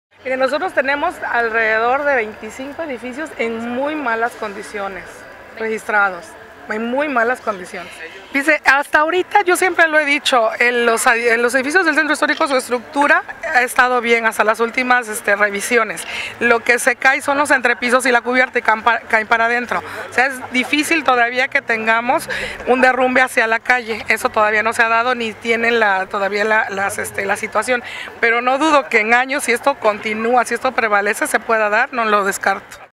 25 edificios del centro histórico permanecen en pésimas condiciones
Carmelina Priego Medina, arquitecta del Centro INAH Veracruz, destacó que al menos 25 edificios del centro histórico de la ciudad permanecen en pésimas condiciones, sin embargo, no representan ningún riesgo para la población a pesar de que por el interior sus pisos y demás se han desplomado.